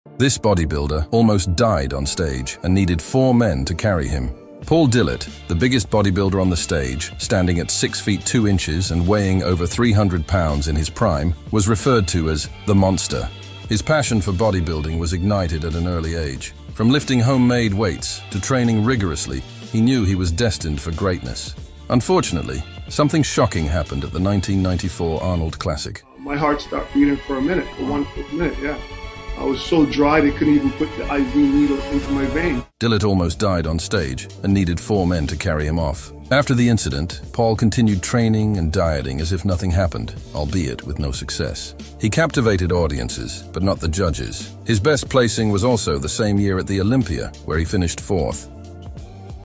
The Day the Giant Fell
Discover the gripping story of Paul Dillett, known as 'The Monster', one of the most formidable figures in the world of bodybuilding. Standing at an impressive six feet two inches and weighing over 300 pounds in his prime, Dillett's journey was marked by ambition, strength, and an unyielding passion for bodybuilding. This video delves into a pivotal moment at the 1994 Arnold Classic, where Dillett faced a near-death experience on stage, requiring the assistance of four men to carry him off. Despite this harrowing incident, he persevered, continuing to train and diet with unwavering determination. Join us as we explore the highs and lows of Dillett's career, his mesmerizing presence that captivated audiences, and his notable achievements, including his best placing at the Olympia in the same year. This is a tale of resilience, the relentless pursuit of greatness, and a reminder of the unpredictability in the world of competitive bodybuilding.